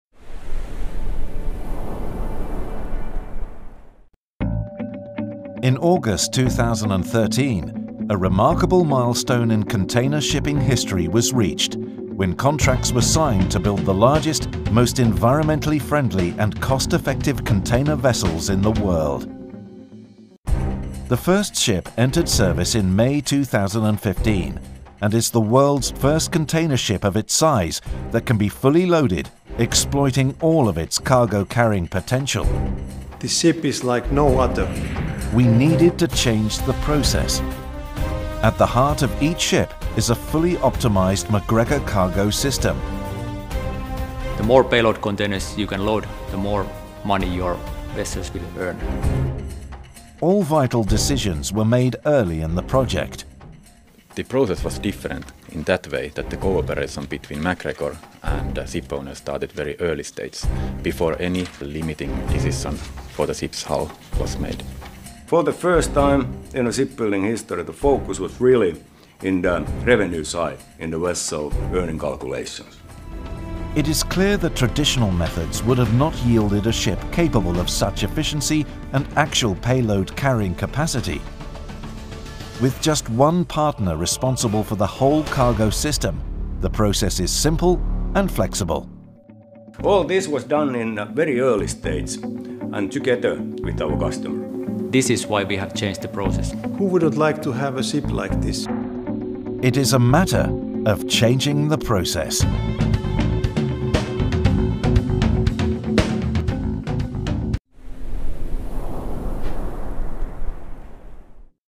MacGregor: Changing the process of container ship cargo system design
Changing the established cargo system design and construction processes requires additional effort but pays off in form of improved cargo system payload capacity. The process needs to start at a very early stage, before any restrictive decisions for the hull have been made to ensure that the design will meet the requirements of the ship’s operation and business case. This approach has proven itself in case of United Arab Shipping Company’s M/V Barzan, the first in series of ultra-large state-of-the-art ships that set a benchmark in the field of eco-friendly engineering and efficiency.